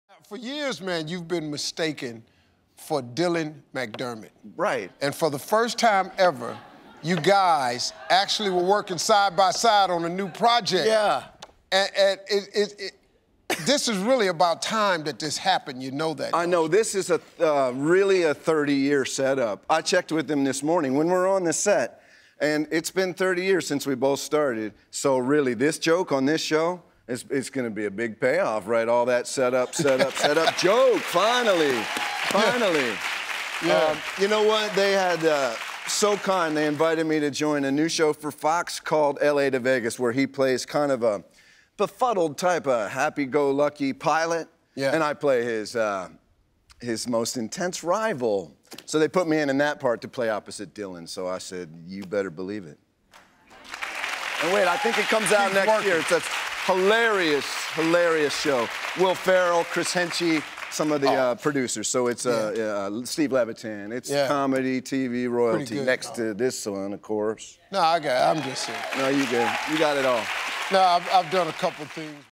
Dermot Mulroney or Dylan McDermott?
Their names get confused. Their faces get mistaken. And it all gets a payoff on their new show ‘L.A. to Vegas’.